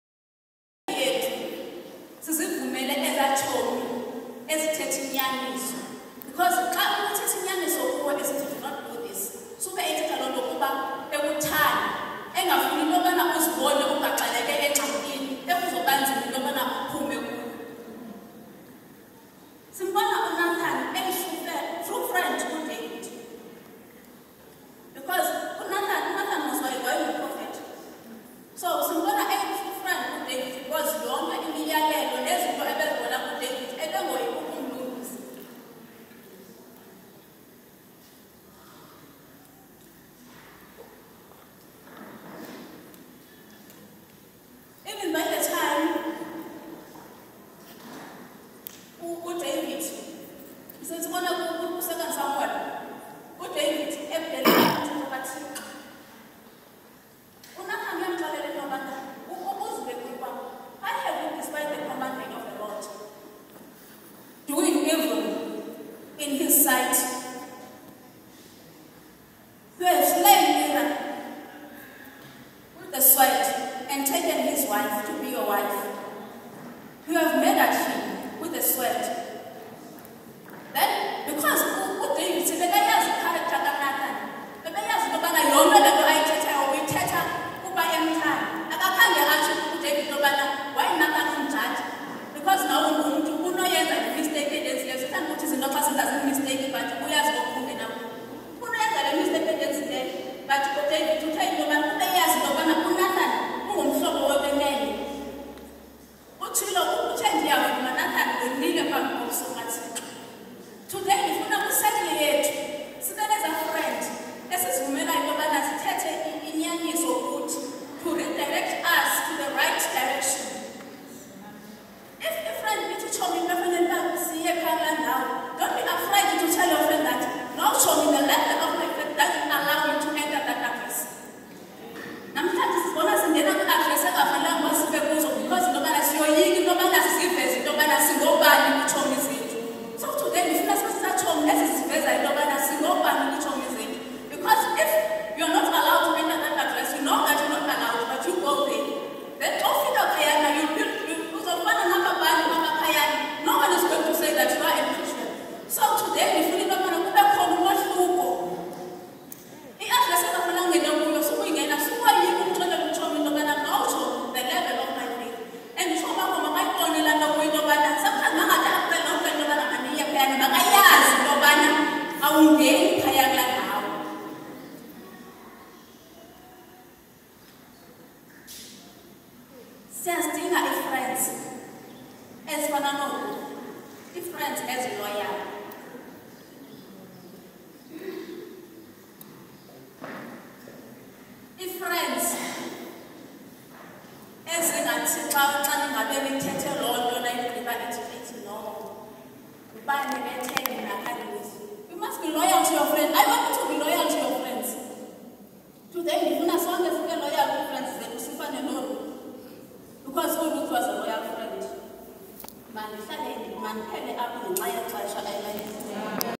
DC2 CP1 Senior Youth Rally 18thNov23 “Your circle of influence” @Ikhwezi Community Center Gugulethu
DC2 CP1 Senior Youth Rally 18th Nov 2023 Theme “Your circle of influence” @Ikhwezi Community Center Gugulethu